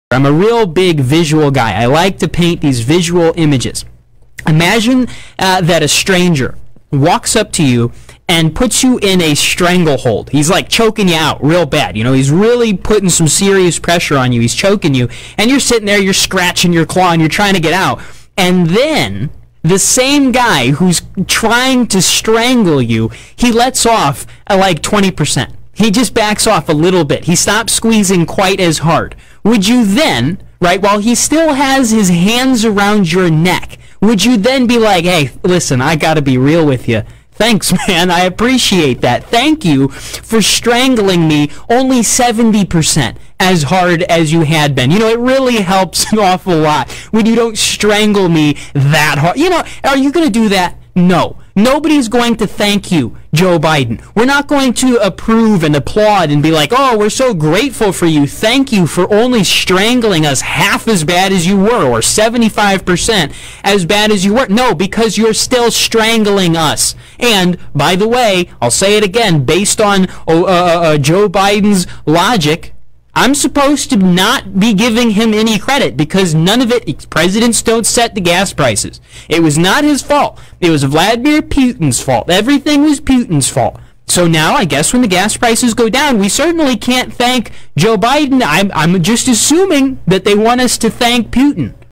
Should We Thank Joe Biden For Lower Gas Prices?
Gas prices have gone down nearly a dollar. A record high DROP in prices.. so who, if anyone, do we thank?